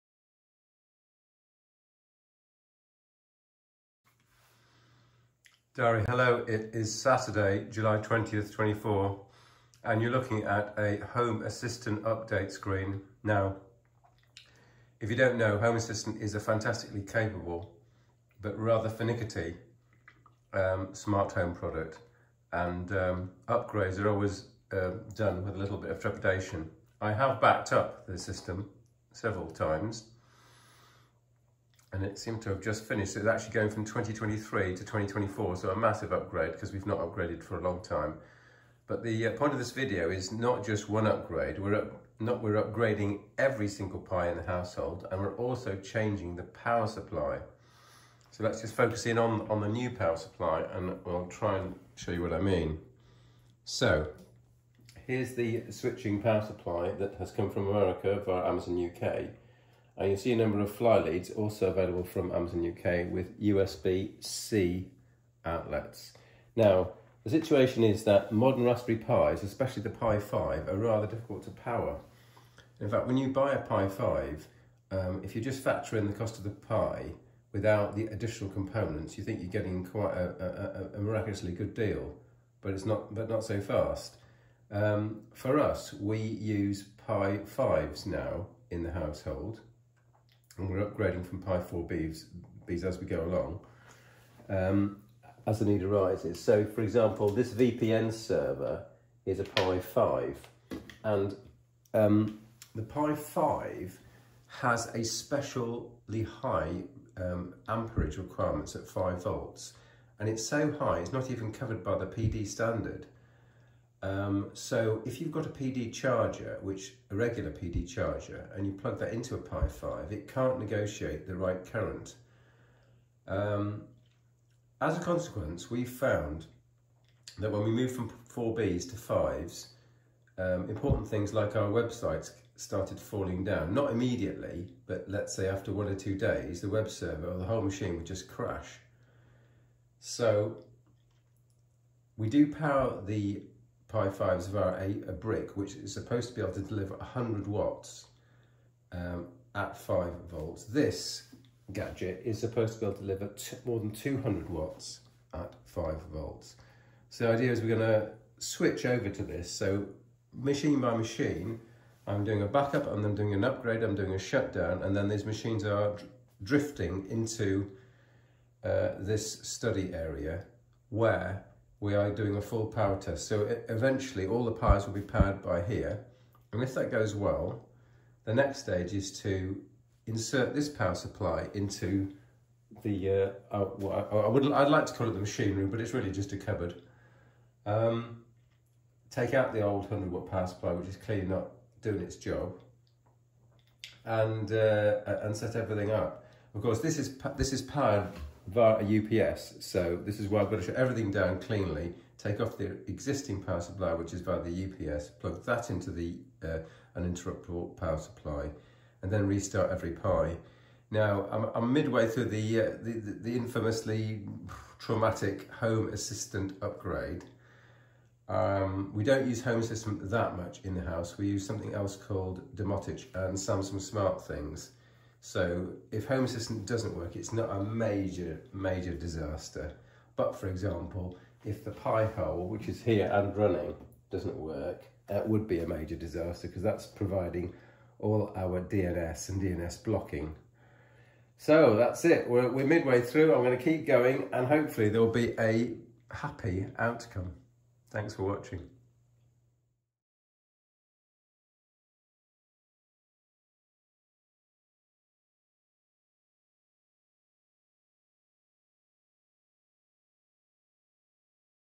20240720 Powering Multiple Raspberry Pi's including 5
After hangs and crashes especially with power hungry Pi 5 systems it was time to implement a professional solution.

Kit Links and words will be at blog article on smpl.fyi when I write it.